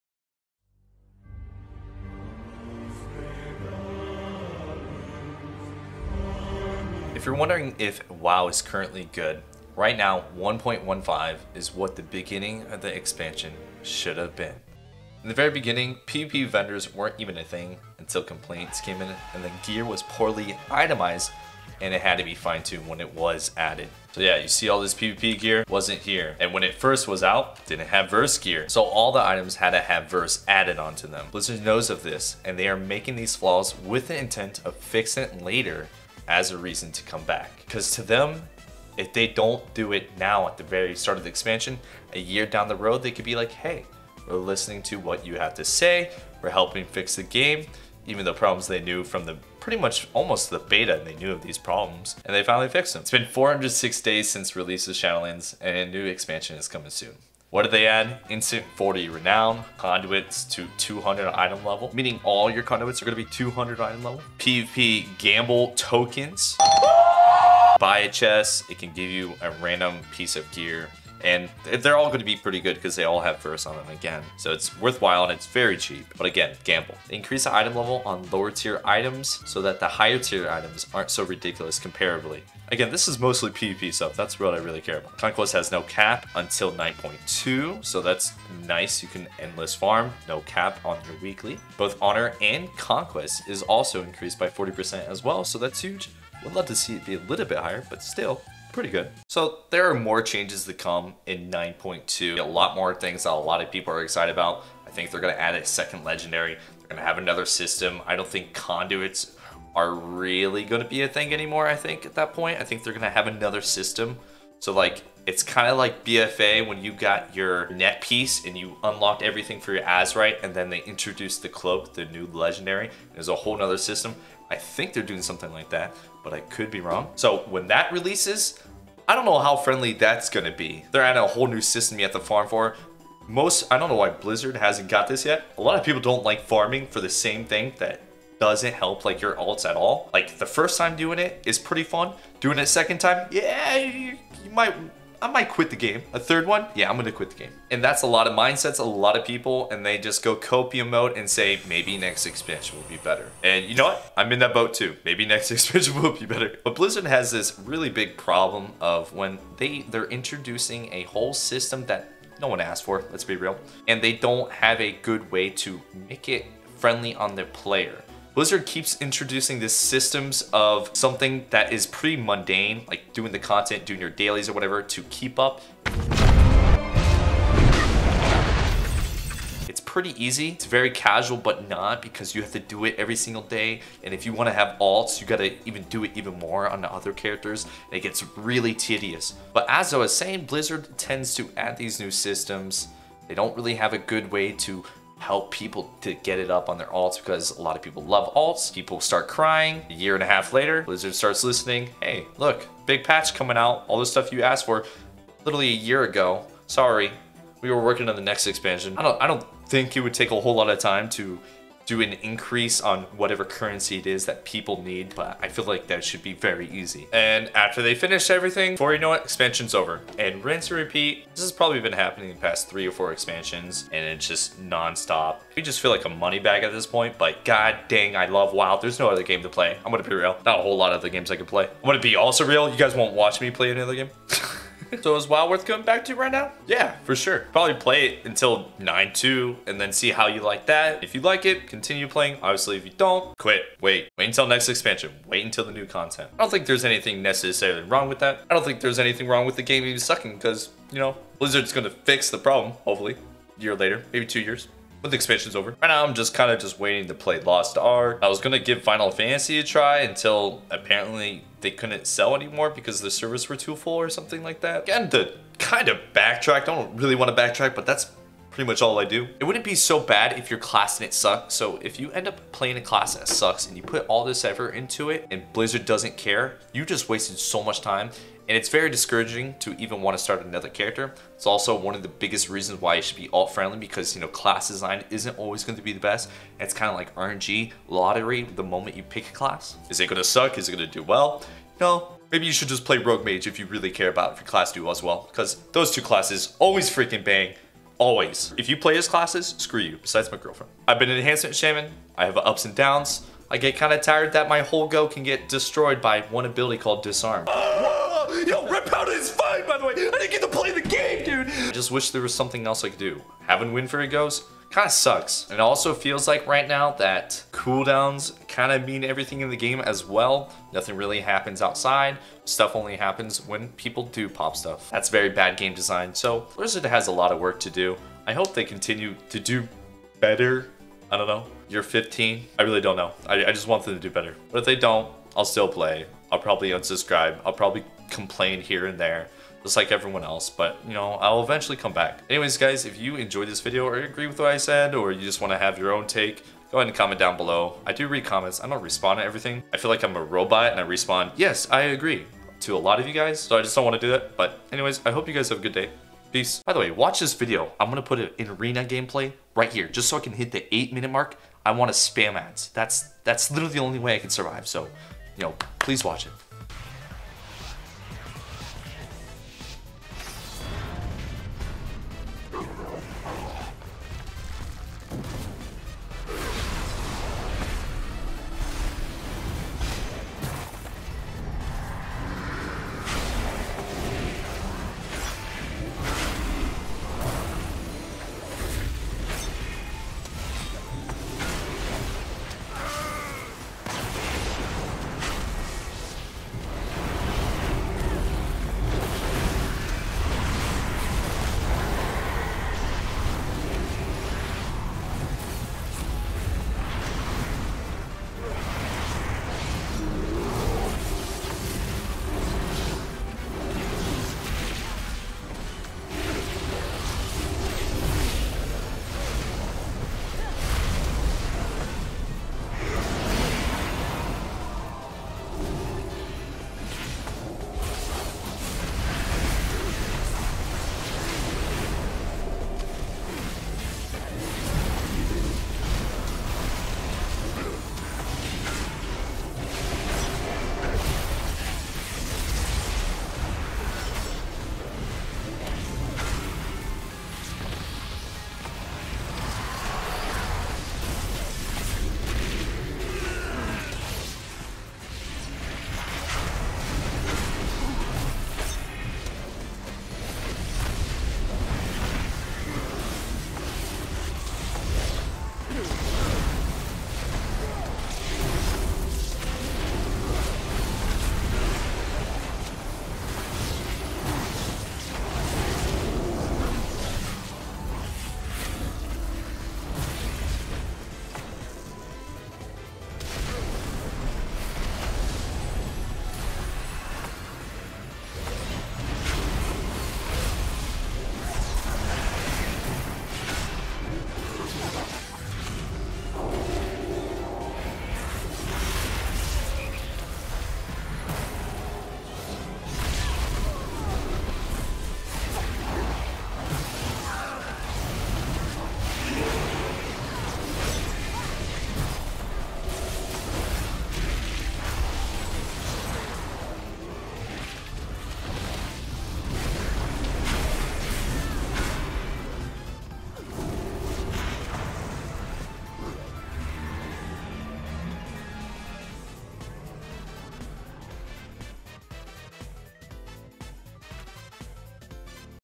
IS WOW WORTH COMING BACK TO??
How 9.1.5 should of been the start of the expansion.